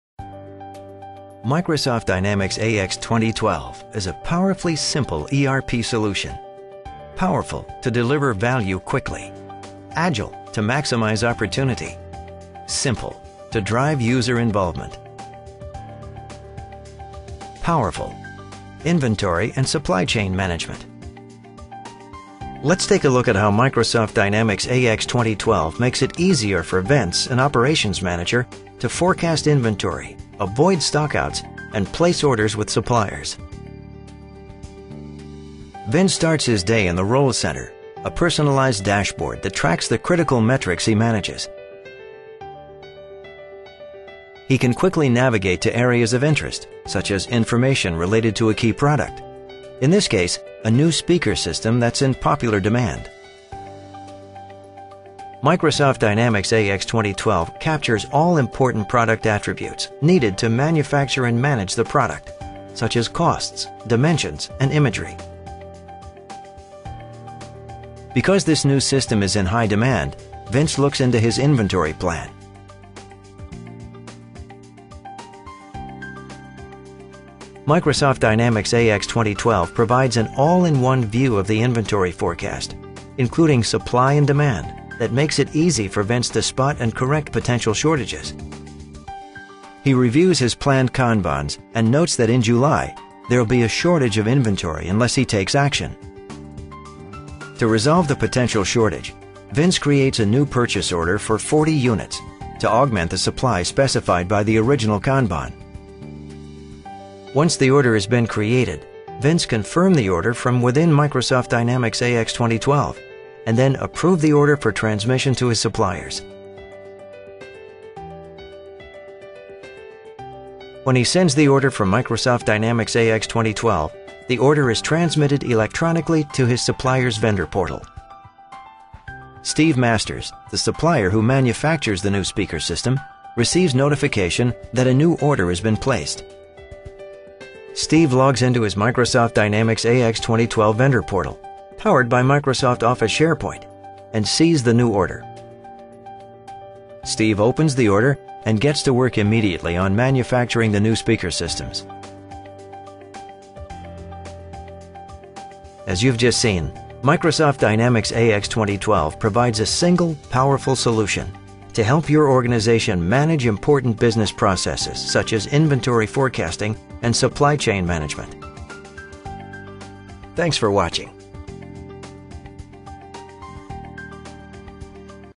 Microsoft Dynamics AX 2012: Supply Chain Management Demo
Take a look at how Microsoft Dynamics AX 2012 makes it easier for operations managers to forecast inventory, avoid stock-outs, and place orders with suppliers. Microsoft Dynamics AX 2012 also provides a single, powerful solution to help your organization manage important business processes, such as inventory forecasting and supply chain management.